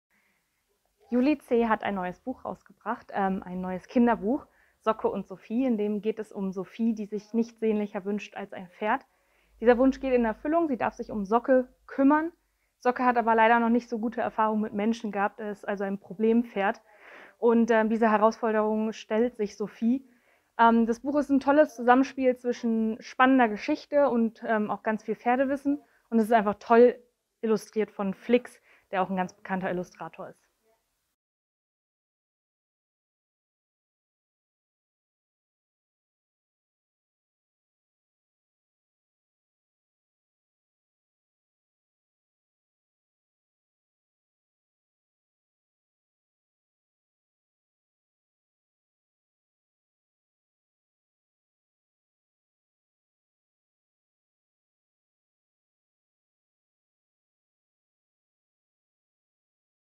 “Socke und Sophie - Pferdesprache leicht gemacht” von Juli Zeh | Böhnerts Buch der Woche
Heute hat unsere Kollegin Imke Geers aus unserer Filiale in Burgwedel für euch das neue Kinderbuch von Juli Zeh mitgebracht, in dem sich Sophie nichts sehnlicher wünscht als ein Pferd und dieser Wunsch geht in Erfüllung - mit Socke, einem Pferd, das noch nicht viel Erfahrung mit Menschen hat.

Frau Geers erzählt, dass das Buch ein tolles Zusammenspiel zwischen spannender Geschichte und ganz viel Pferdewissen - und wurde wunderschön illustriert von Flix, dem bekannten Illustrator.

“Socke und Sophie - Pferdesprache leicht gemacht” von Juli Zeh
Kinderbuch, Empfohlen 10 bis 99 Jahre,  aus dem dtv-Verlag.